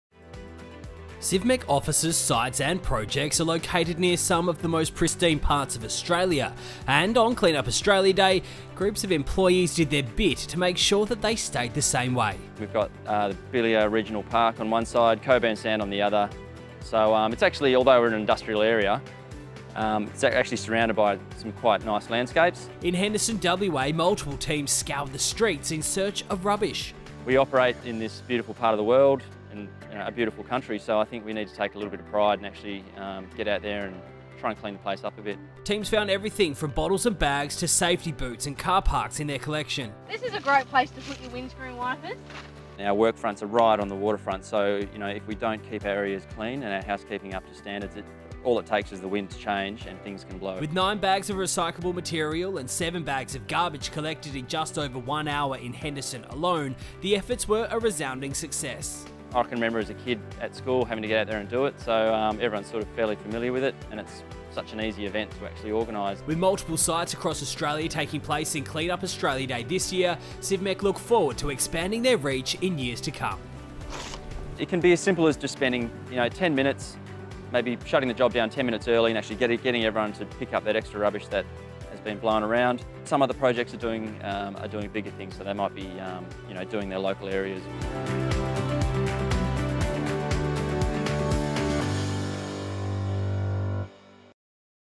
Clean Up Australia Day 2018
Civmec's employees at our facilities and projects yesterday participated in Clean up Australia Day - helping to maintain the pristine areas clean.